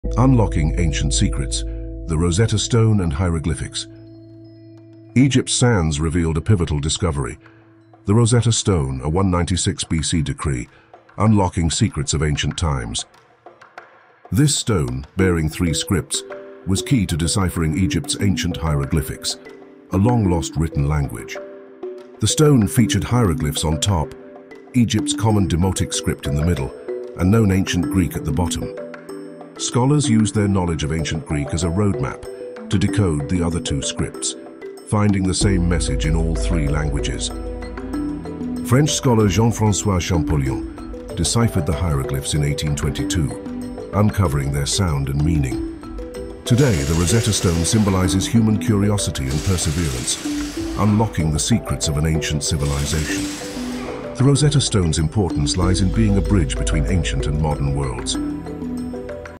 The Rosetta Stone: Deciphering Ancient Languages
The Rosetta Stone: Deciphering Ancient Languages' delves into the pivotal role the Rosetta Stone played in deciphering Egyptian hieroglyphics. This short highlights its significance in unlocking the secrets of ancient civilizations and how it served as a crucial key in understanding the rich history of ancient Egypt.